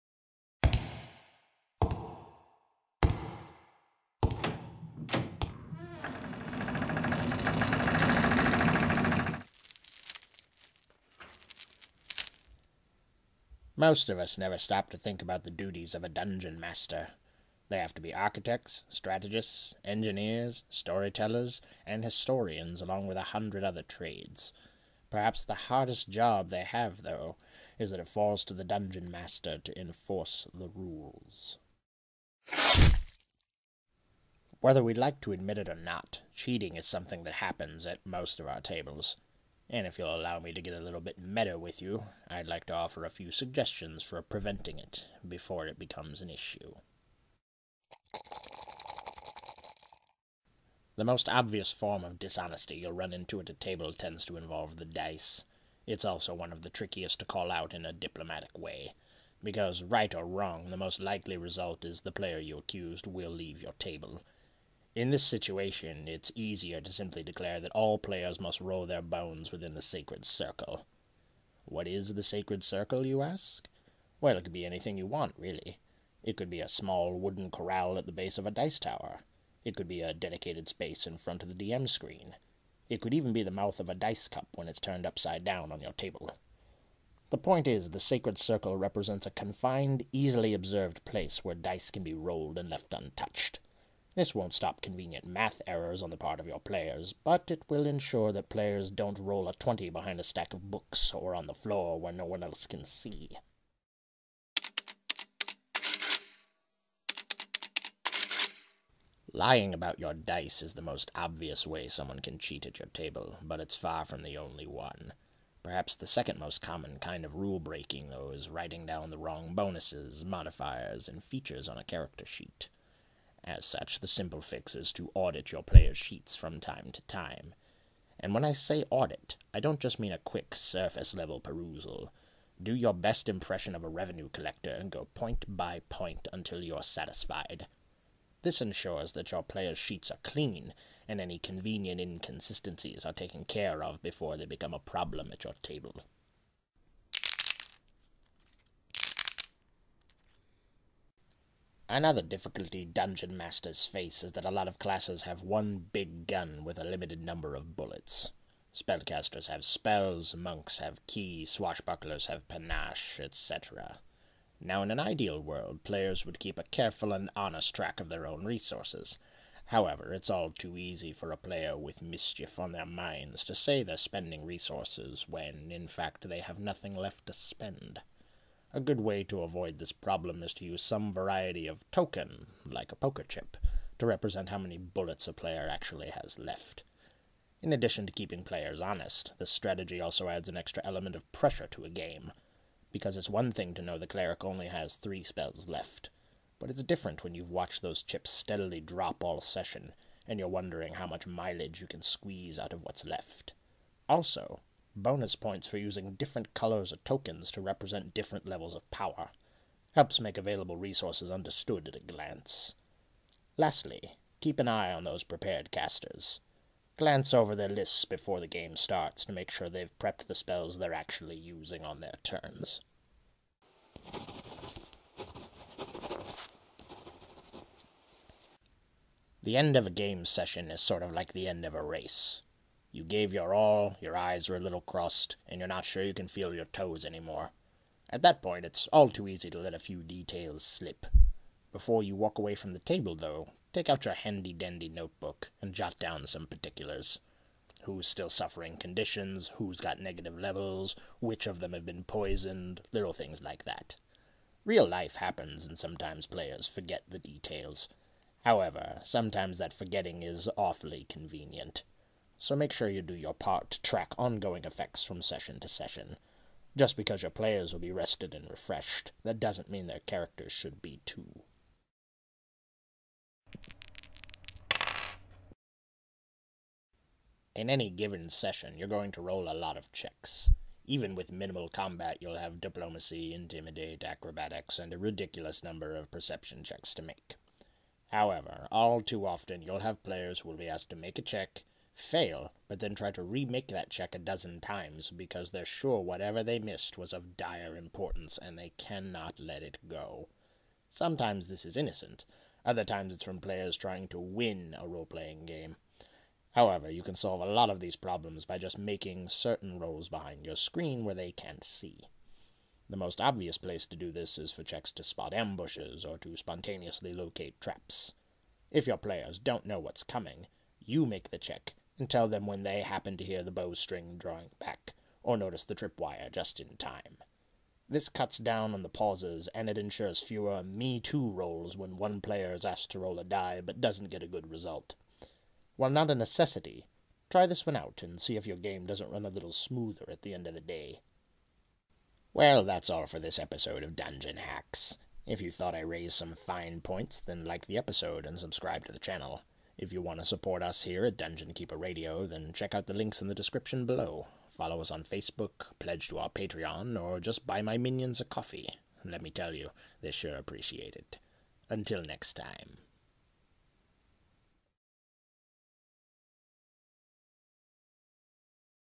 Dungeon Hacks 5: 5 Ways To Prevent Cheating At Your Table
There are some challenges every dungeon master must face in their time behind the screen. Cheating is one of them. Forewarned is forearmed, though, so listen to the Dungeon Keeper's simple methods for preventing this kind of dishonesty from happening at your table in the first place.